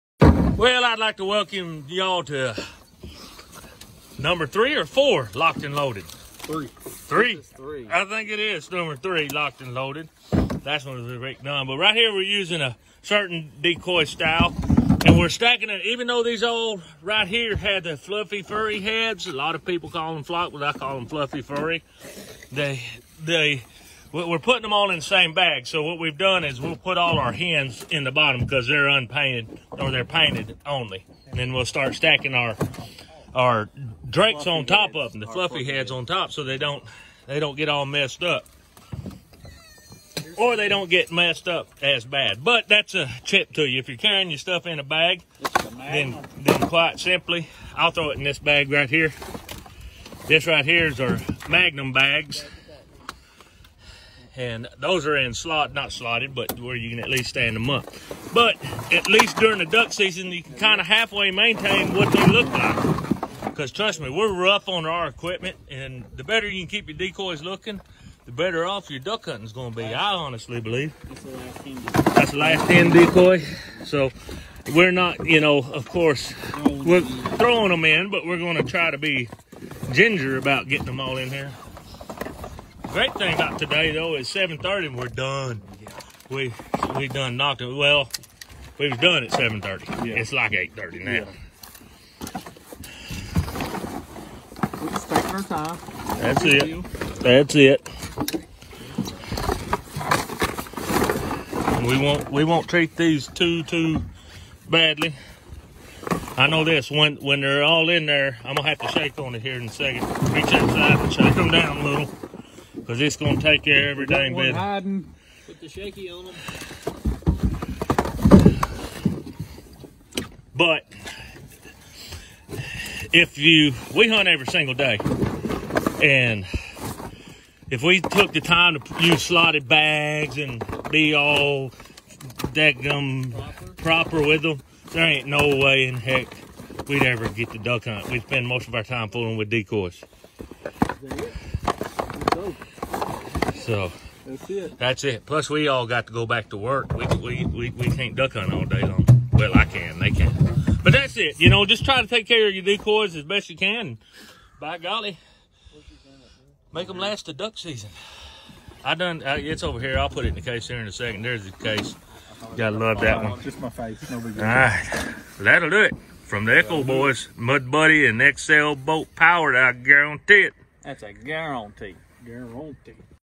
Dealing With Decoys - Locked & Loaded - 4
We ain’t careful by no means with our decoys but we do try to keep them somewhat good looking thru the season.